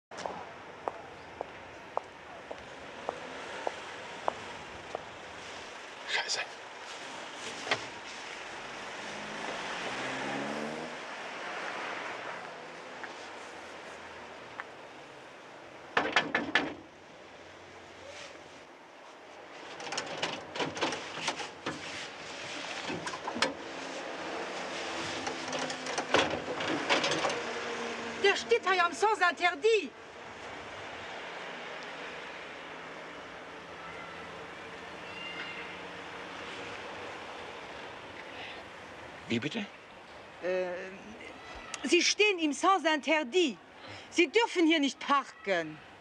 Dir stidd am Sens Interdit
Eng Peschtert schwätzt den Erwin un, deen op den Moreno a Guddebuer ward, déi grad eng Bank iwerfaalen.